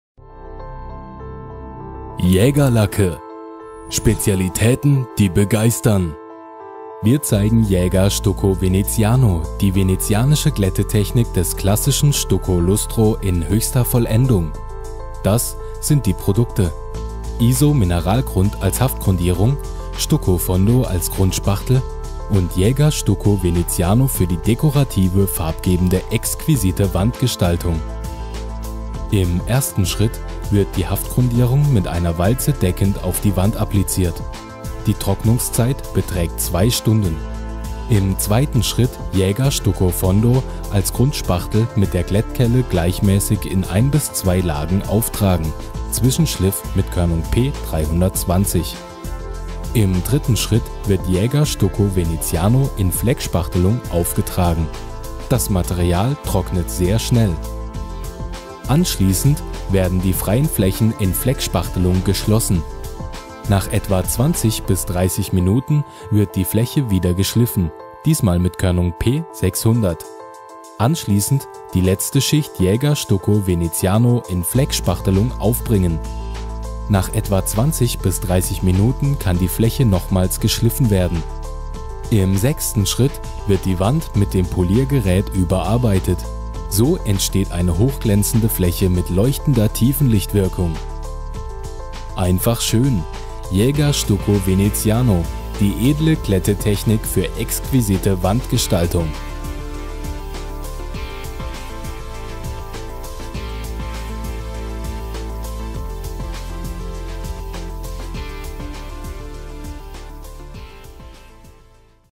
Villa Venezia: Stucco Veneziano Verarbeitungsanleitung
Die Verarbeitungsvideos der Jaeger Villa Venezia Kreativtechniken zeigen Schritt für Schritt die sach- und fachgerechte Verarbeitung der edlen  Maltechniken im mediterranen Stil gezeigt. Auch die  Untergrundvorbehandlung wird genauestens beschrieben. Das Video Stucco Veneziano ist eines von fünf Verarbeitungsanleitungen der Videoreihe Villa Venezia und beschreibt das Original für die klassische venezianische Glättetechnik.